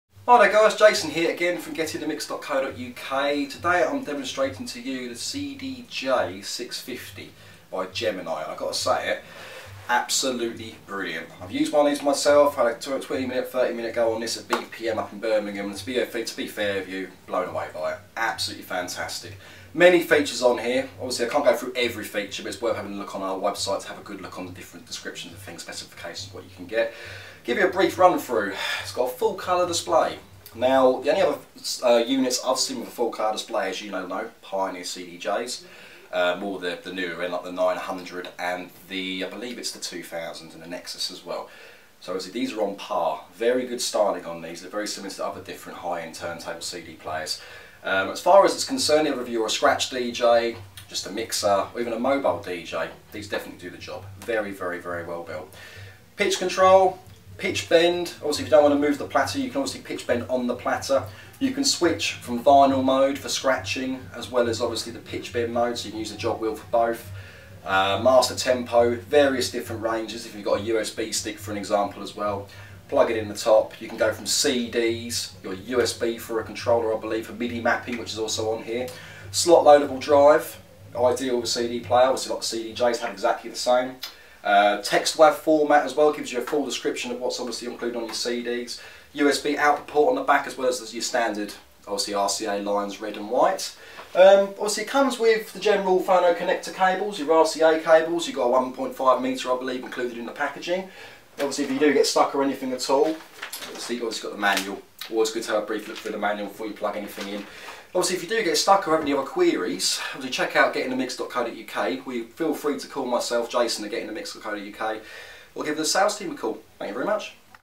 Gemini CDJ650 DJ CD Player - Demonstration
Gemini CDJ-650. The Gemini CDJ-650 is a sturdy professional table top DJ media player full to the brim with clever features. The Gemini CDJ-650 has got it all and more. It has a slot-in CD drive so no CD tray to get in the way why you're throwing shapes behind the decks, and not only that, you don't even have to use Cd's as the Gemini CDJ-650 plays your tracks straight from a USB device and even doubles up as a midi controller so you can control the software of your choice straight through the CDJ-650's themselves!